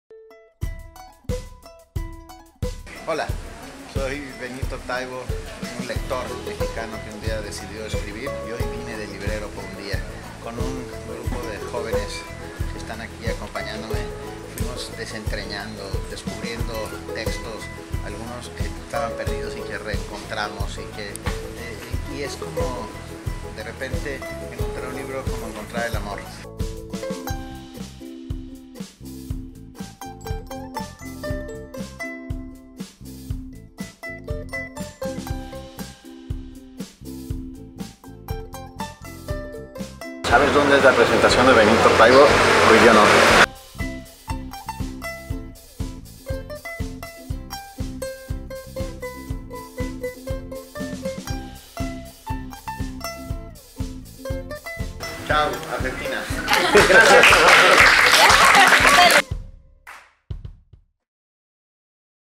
Benito Taibo en Argentina
Benito Taibo vino a Argentina a presentar "Corazonadas" y en su visita fue Librero por un día en Cúspide, almorzó con BBBs y presentó su libro en Grandes Libros.